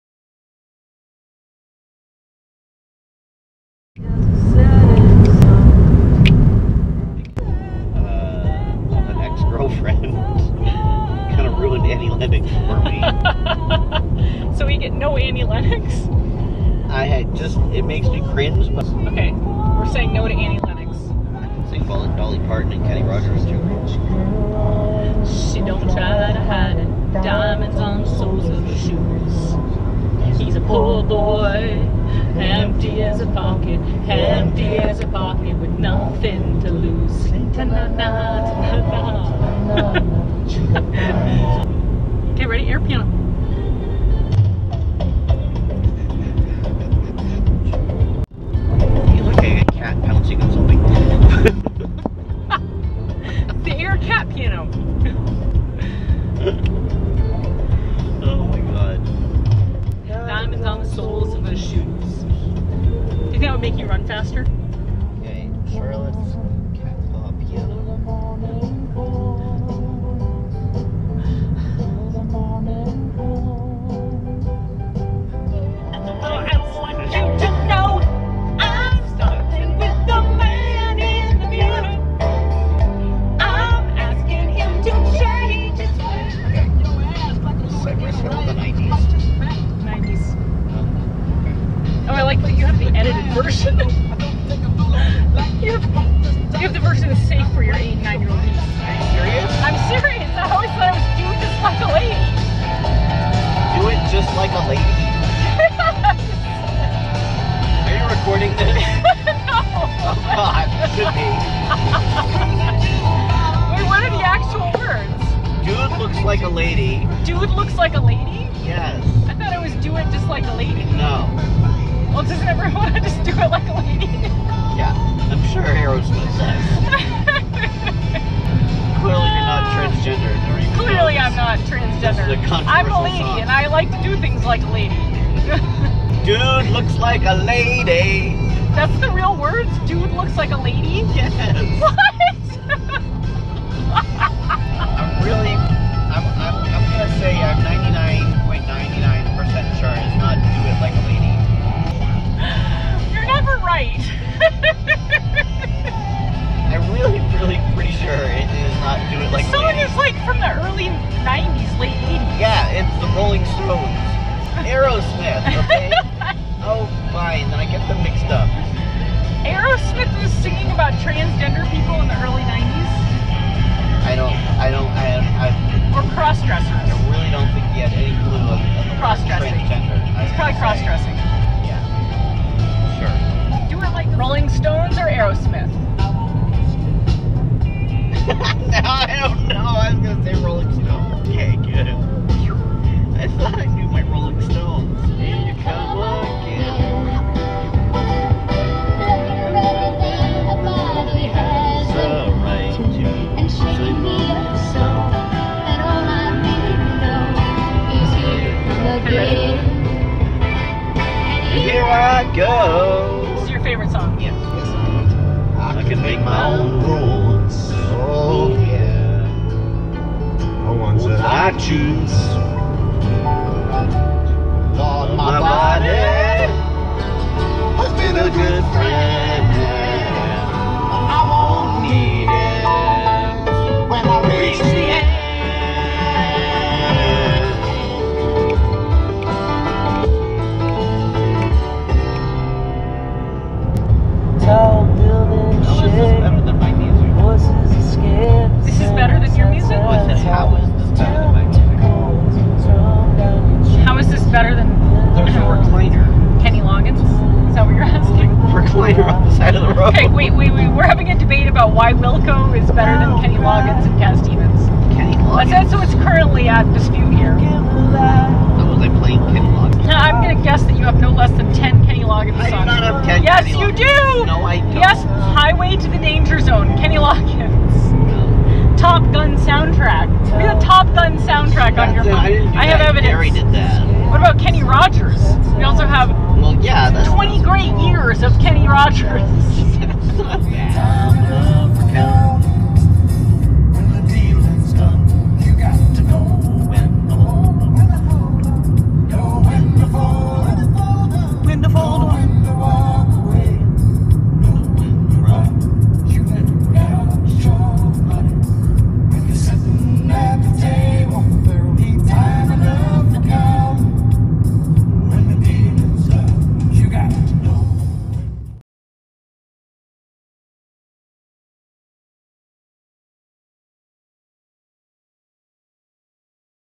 Identical Transgender Twins Road Tripping across Arizona
My brother and I are identical transgender twins. We are always asked how we can be identical if we are different genders, but that's just how the chips/chromosomes fell. This February we met up in Arizona for our 41st birthday to run a trail race together. The road trip to the race involved singing a lot of really bad music.